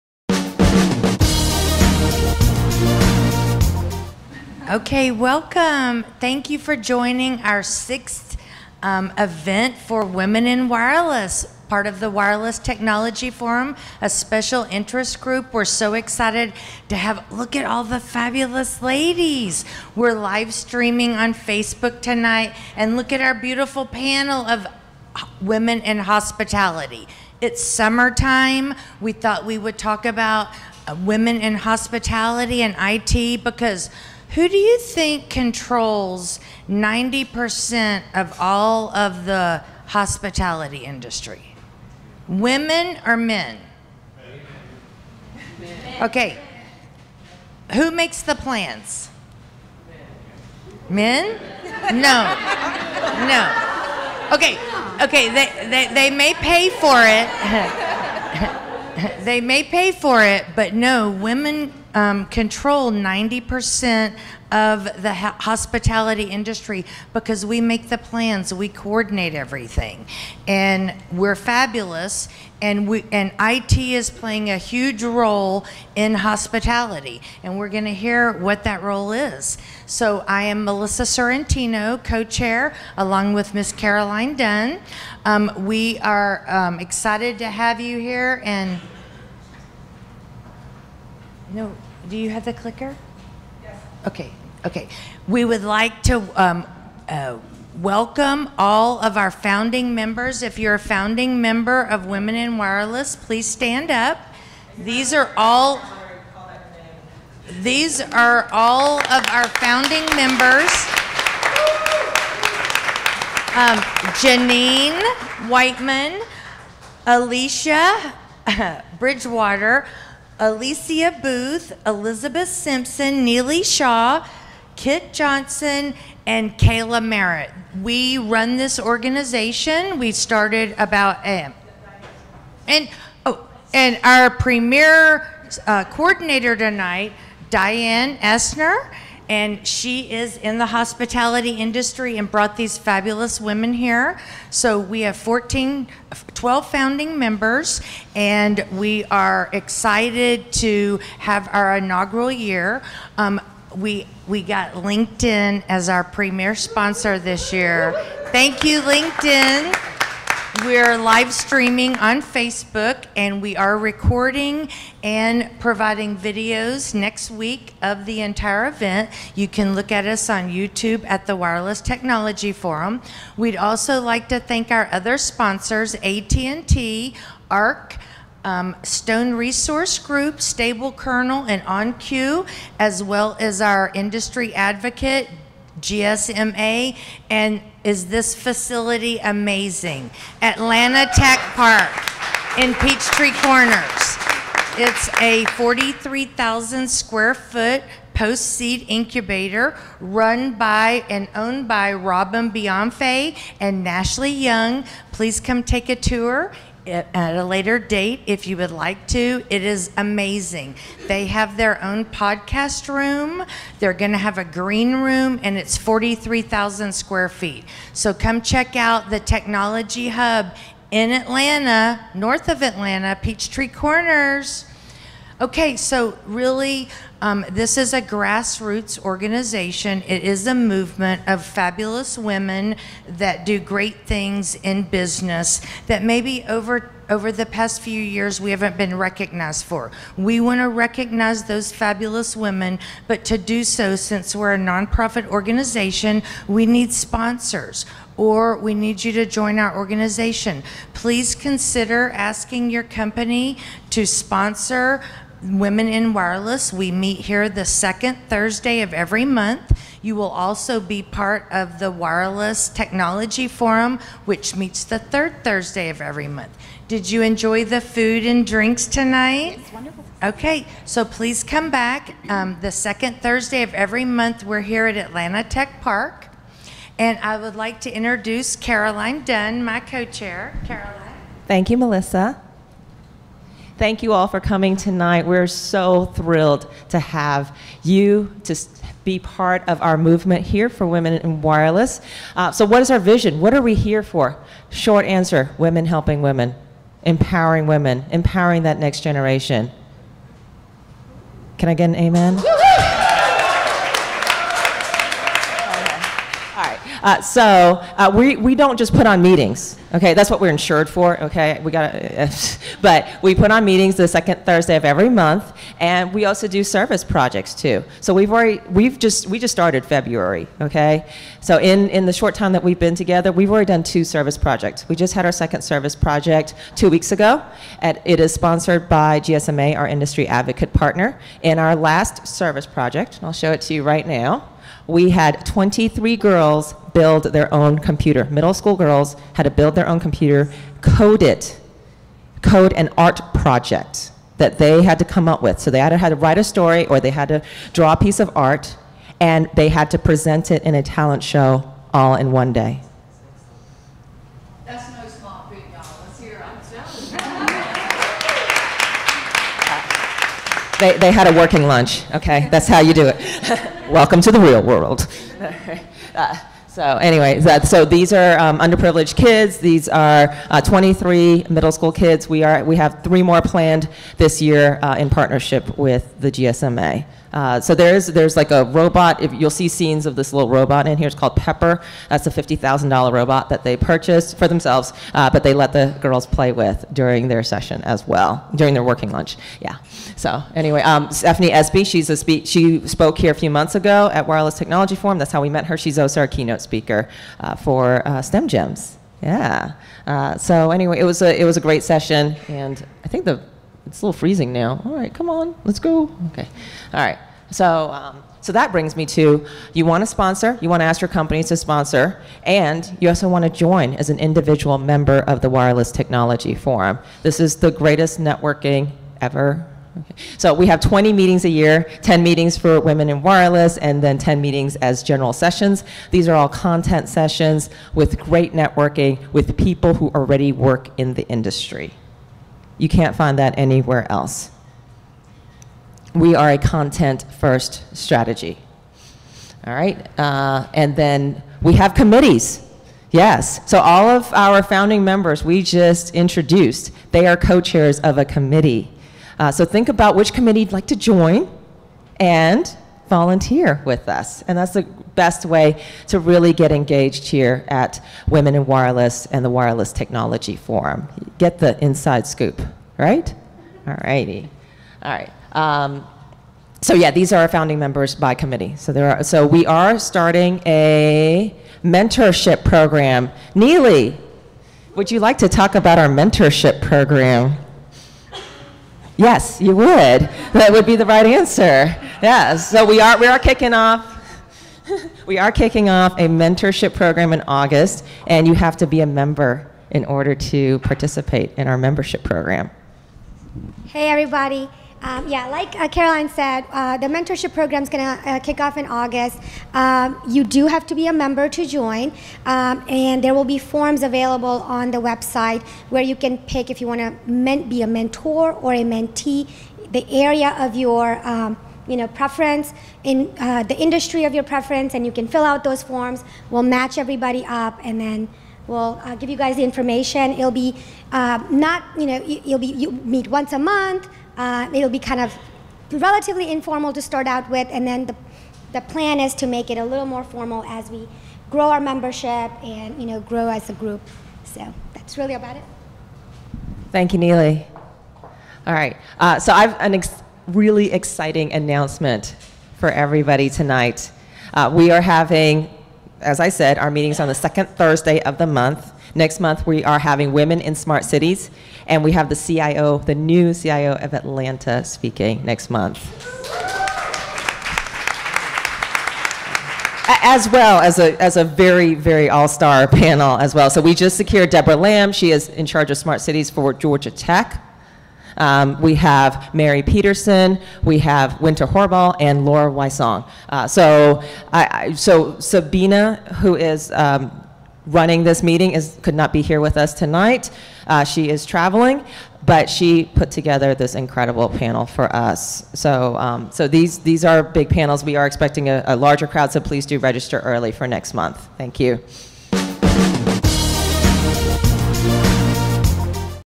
Opening - Women in Hospitality - Women in Wireless SIG - July 2018
Women in Hospitality

Hospitality & travel is the world’s largest industry. It’s about people, culture, diversity, meeting the bottom line, creating uniquely personal experiences, learning and managing dynamic changes, establishing leadership in a man’s world, and keeping a life balance.

Speakers:
Di Davillas, Area VP - Global Accounts, Oracle
Pam Angelucci, President, Hospitality Technology Consulting
Emma Hind, Director, Global Revenue Management Transformation, IHG
Moderator: Amanda Chivers, Managing Principal, Crown Hospitality

Location: Atlanta Tech Park, 107 Technology Pkwy NW, Peachtree Corners, GA 30092